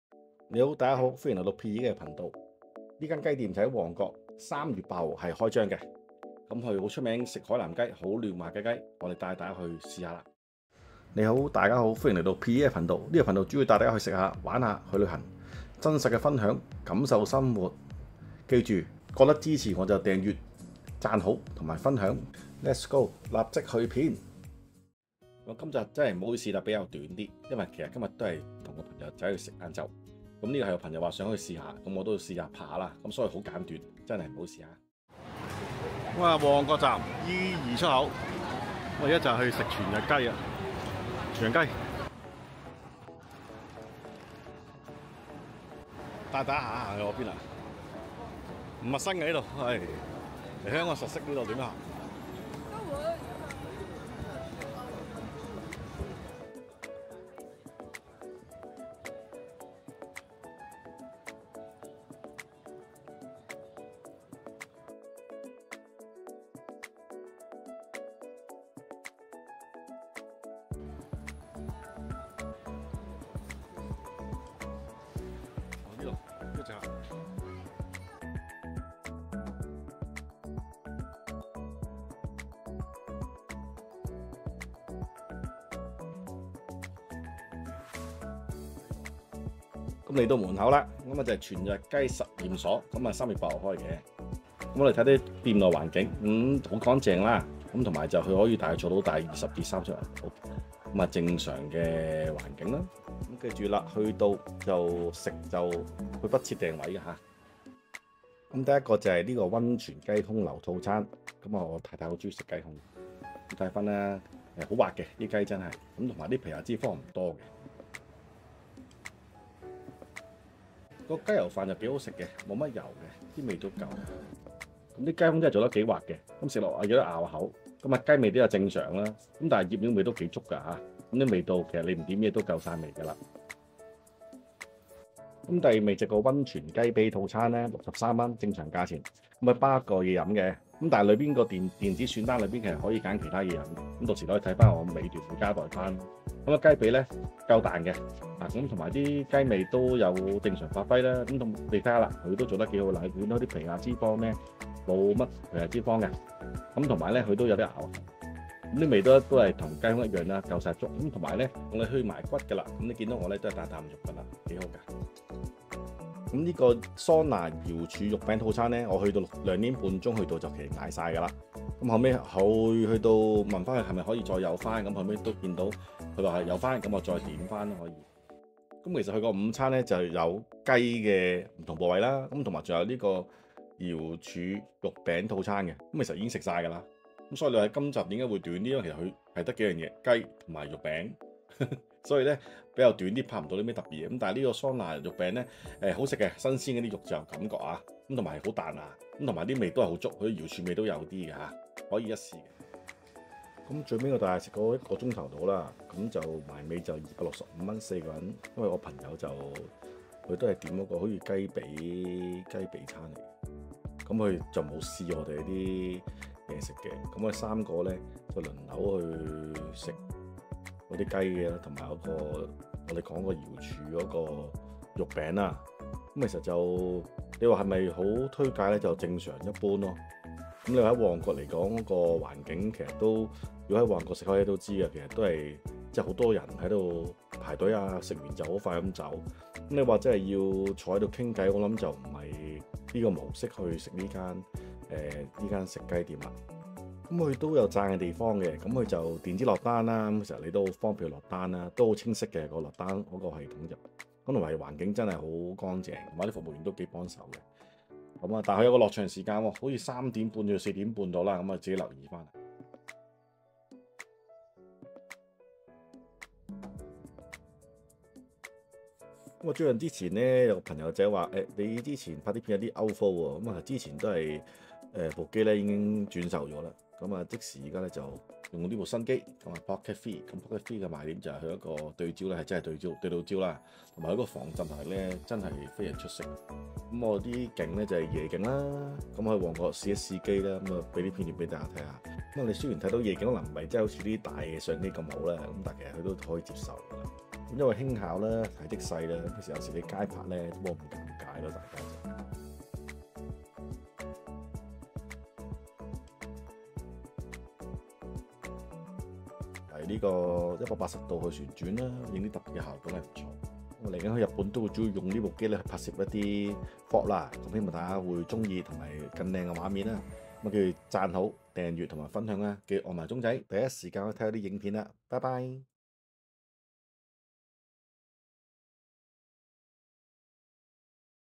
[香港美食探店 海南雞飯] 排隊店 | 24年3月8日旺角開張| 必食嫩滑溫泉雞胸柳|啖啖肉無骨溫泉雞髀 |桑拿瑤柱肉餅 | 用心製作! 分享美味😊!自費食評!4K畫面! 提供字幕!
全日雞實驗所(旺角店)

地址:旺角通菜街80號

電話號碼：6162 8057
營業時間:11:30-21:30 (下午有落場時間)

時間軸：
0:00 開場
0:36 去店路線
1:30 全日雞食店
3:21 總食評
4:25 dji osmo pocket 3 試用示範